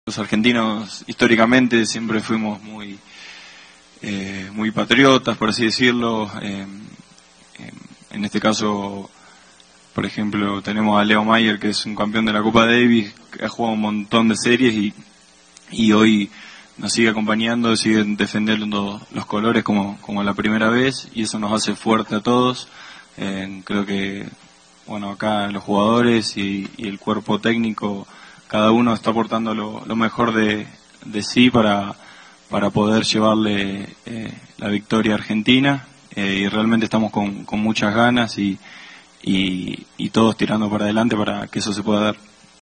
Bagnis Bagnis elogió abiertamente a Leo Mayer
El argentino le tiró flores a su compatriota en la previa a lo que será el cruce con Colombia. Mirá el video.-